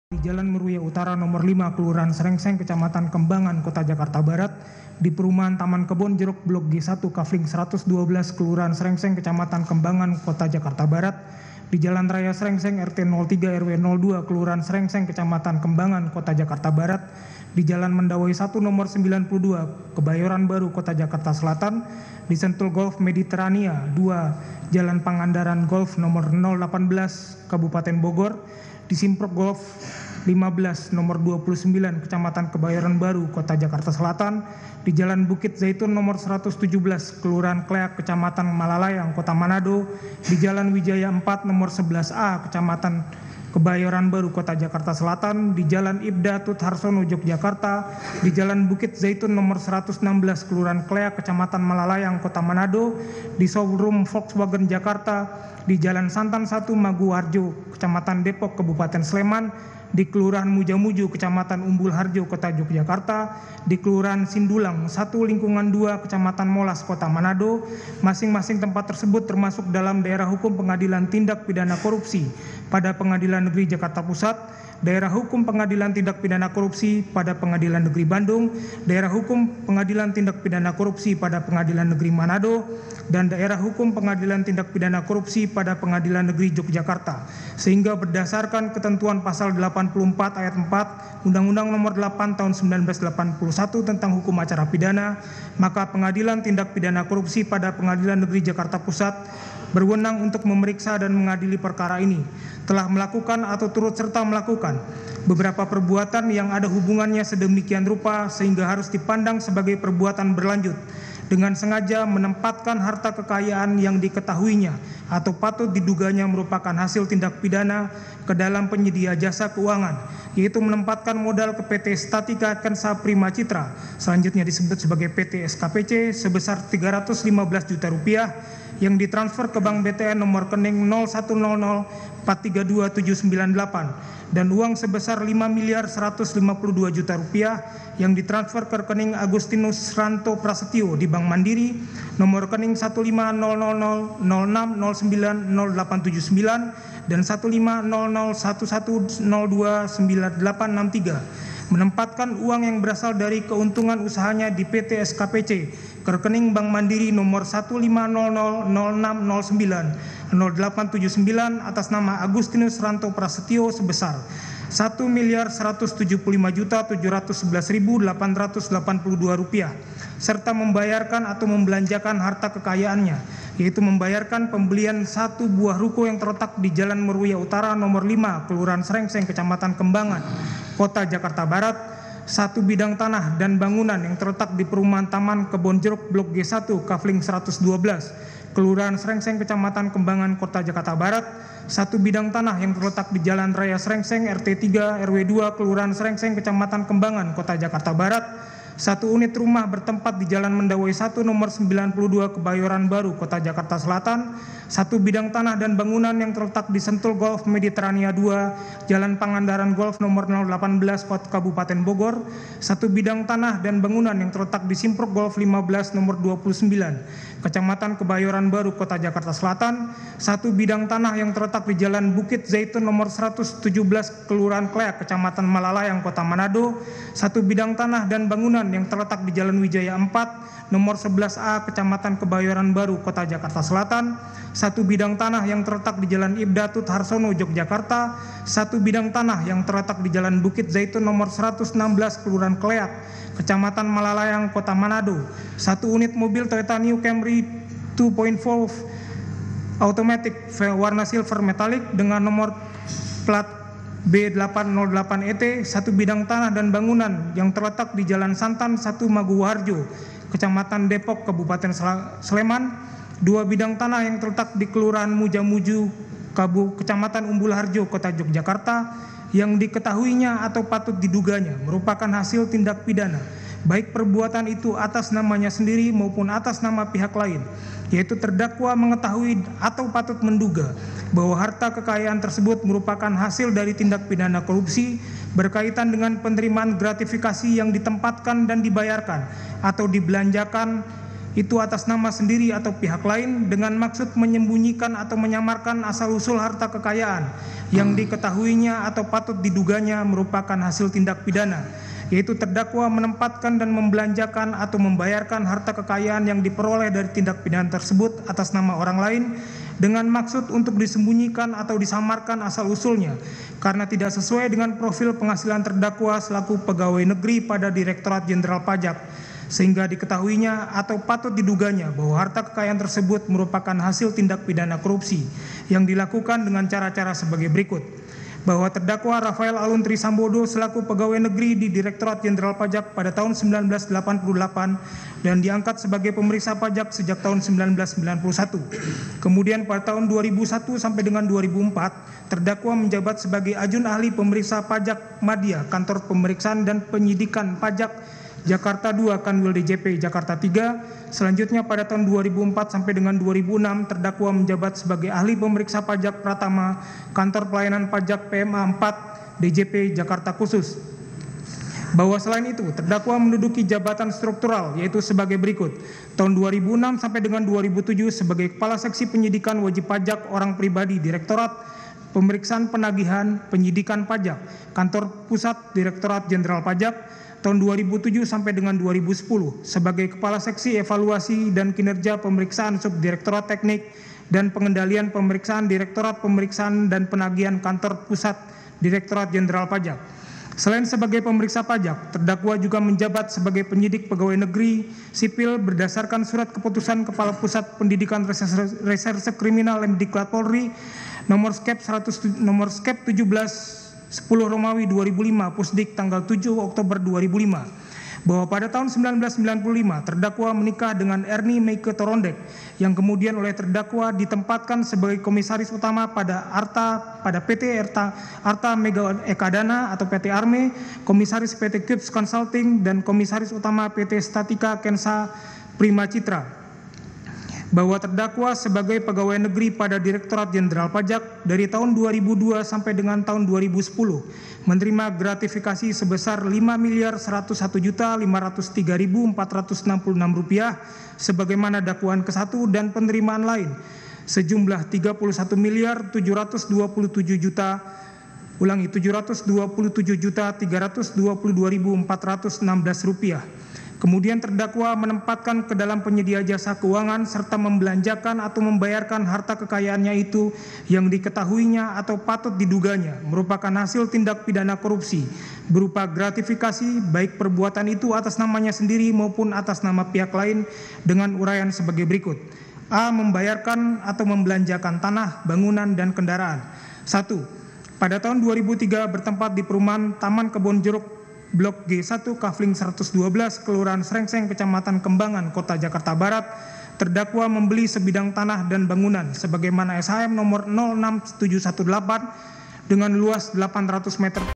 Rafael Alun Beli Rumah di Bandung Hingga Manado dari Uang Korupsi!
KOMPAS. TV - Rafael Alun diduga membeli berbagai properti, seperti rumah dan tanah di berbagai lokasi termasuk Bandung dan Manado, dengan menggunakan uang hasil dari korupsi. Properti tersebut terletak di berbagai tempat di Jakarta, Bogor, Yogyakarta, dan lainnya.

Rafael Alun diduga menempatkan uang hasil tindak pidana ke dalam penyedia jasa keuangan dan membelanjakannya untuk membeli properti, termasuk tanah, bangunan, dan kendaraan.